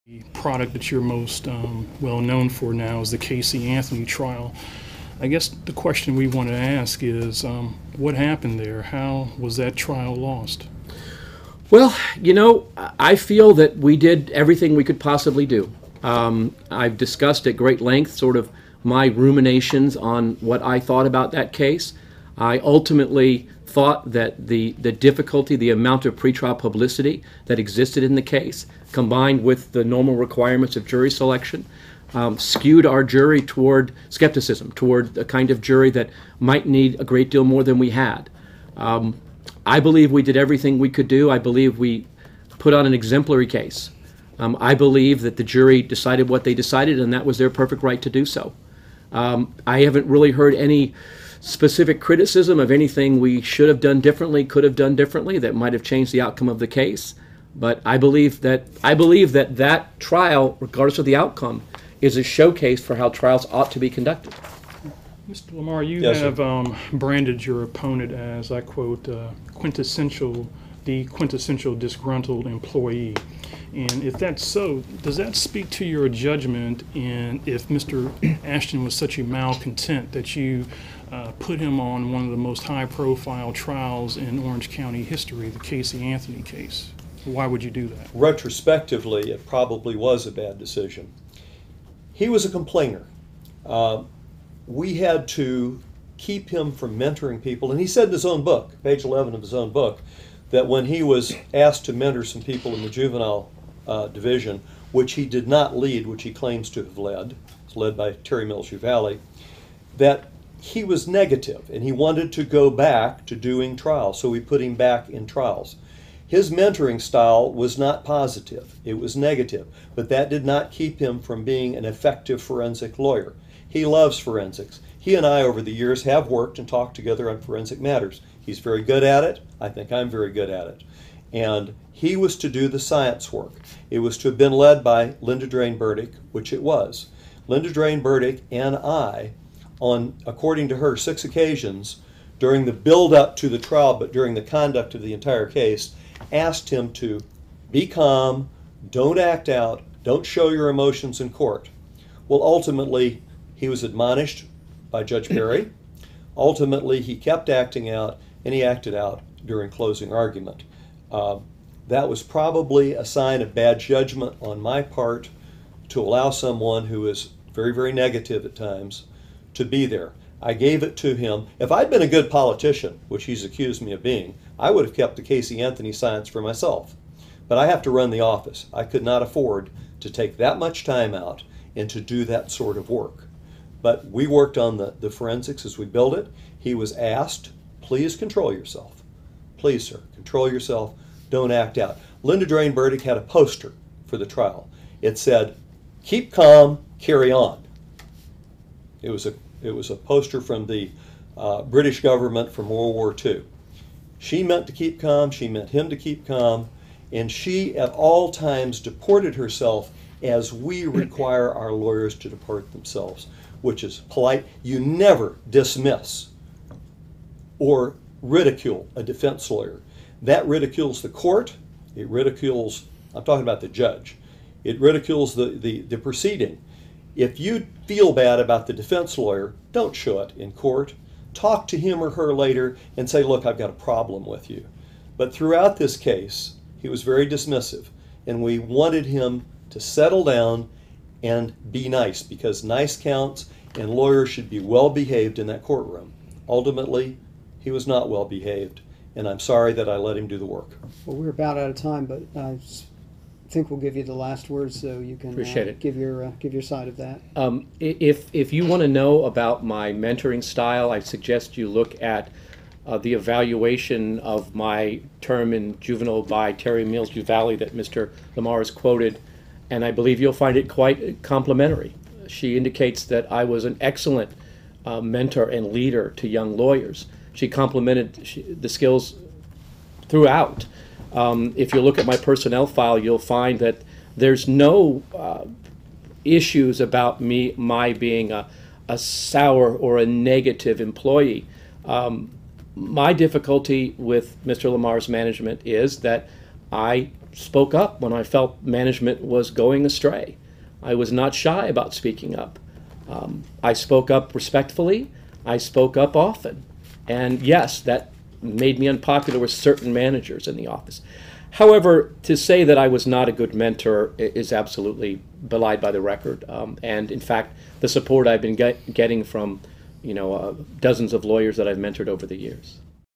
Jeff Ashton, Lawson Lamar reflect on Casey Anthony case
Jeff Ashton and Lawson Lamar reflect on Casey Anthony case during an Orlando Sentinel Editorial Board interview for State Attorney candidates.